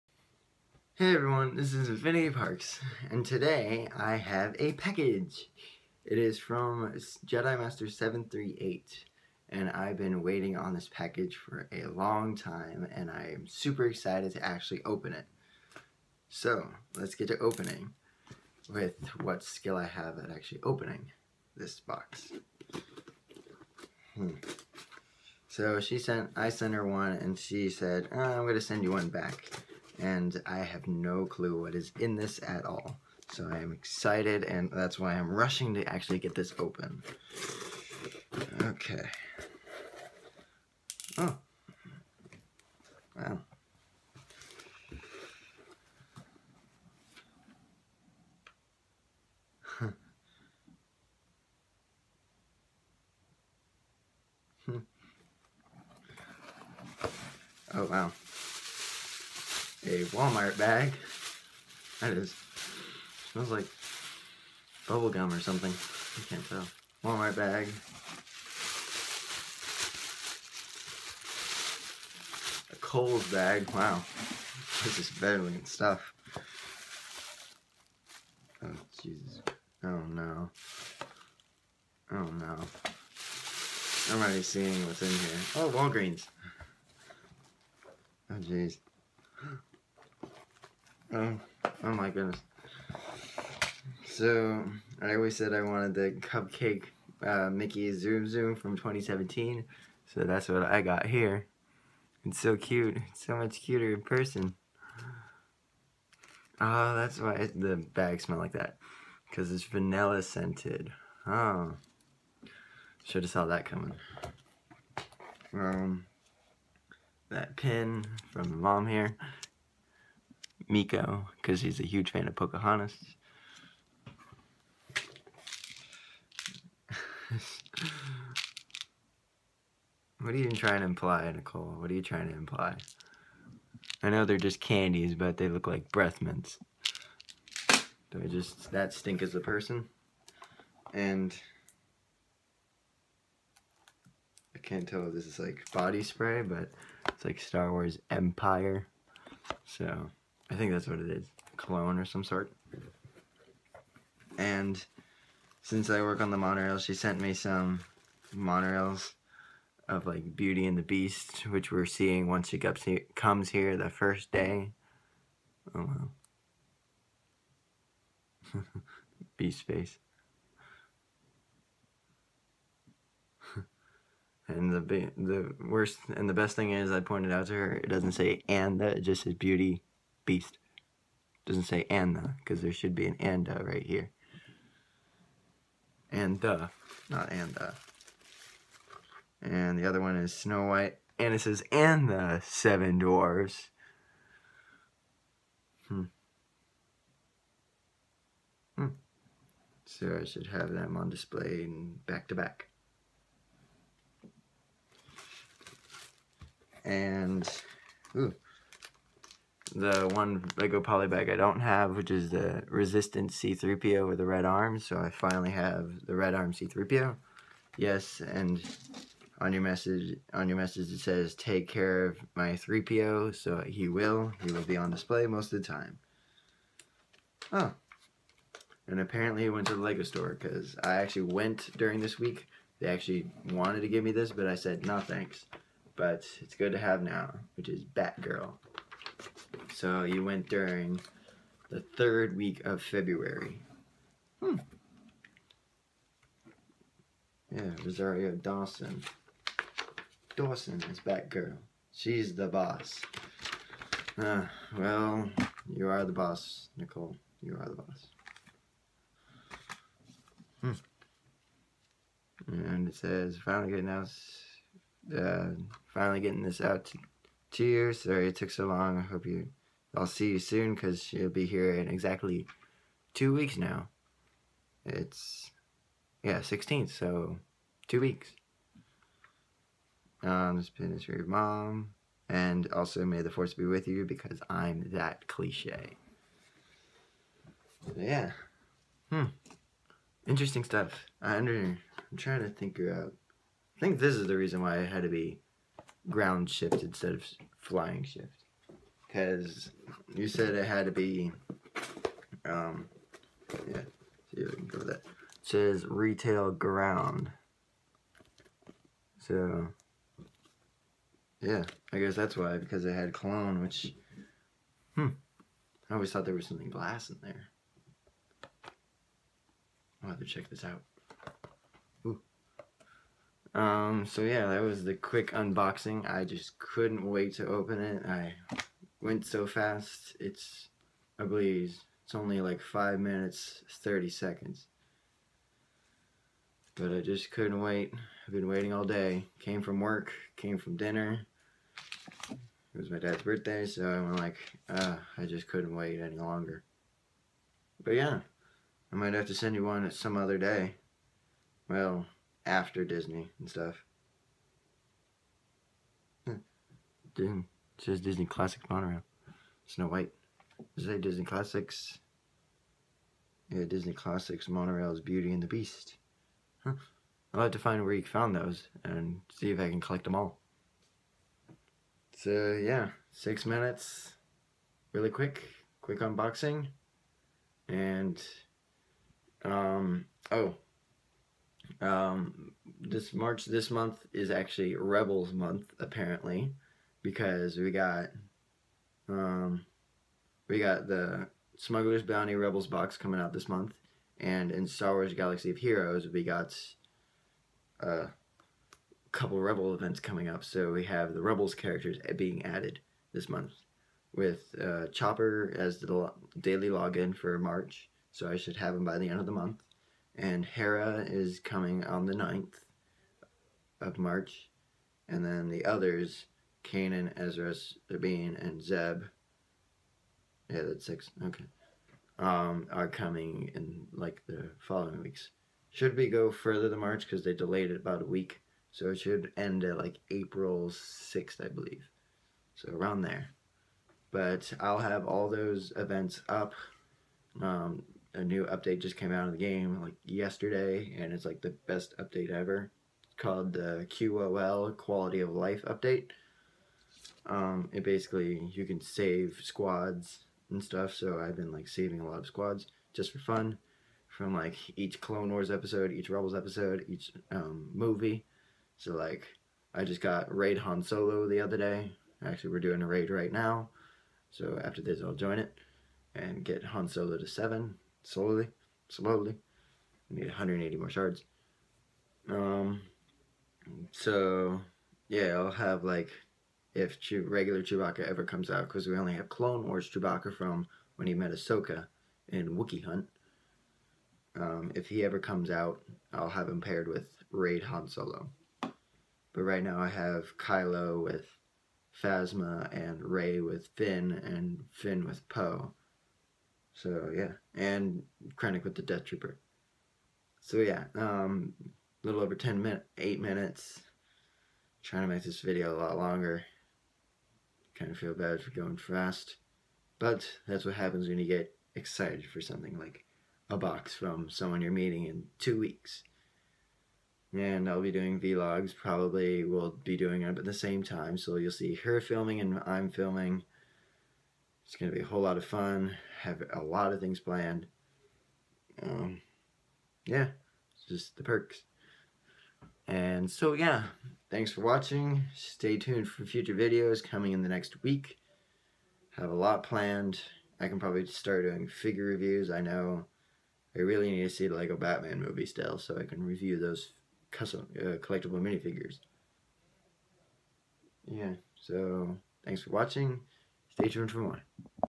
Suprise Unboxing from JediMaster738 + Rebels in March
I got a Package from JediMaster738, who arrives here in 11 days!